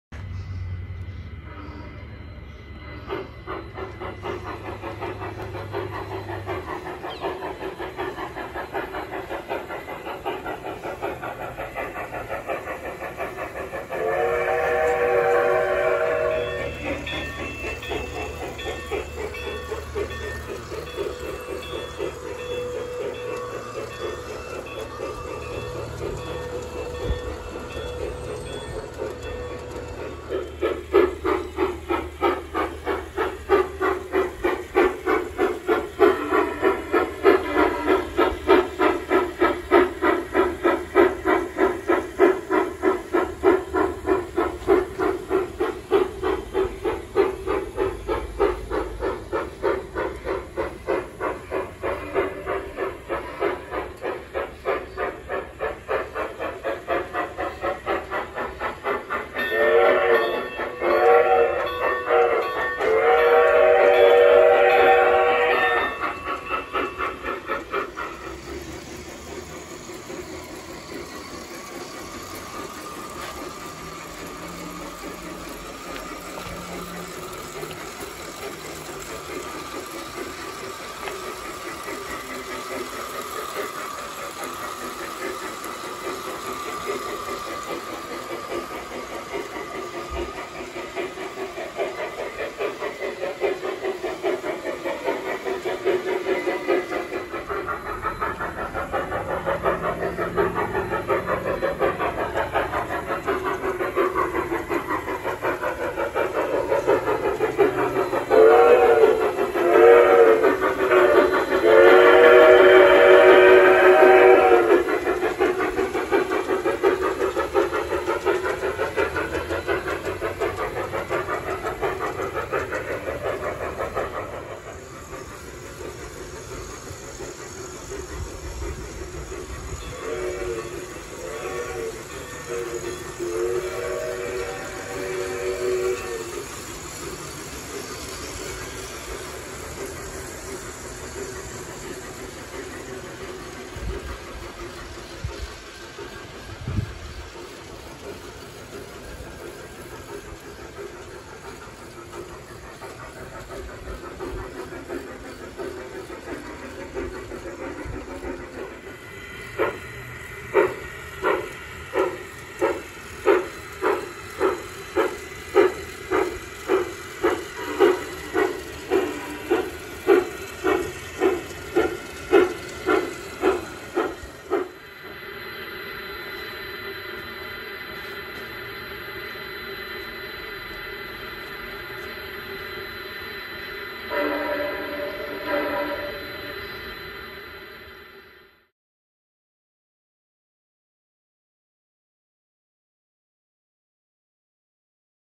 Piko Mogul and C-16 Napa Velley Wine Train
Upland Trains installed some LED lights and little people in my USA Trains Napa Valley Wine Train.  It is pulled behind an Aristocraft C-16 locomotive. Ran it today with the PIKO Southern Pacific passenger train pulled by a Mogul. Kenny certainly does good work.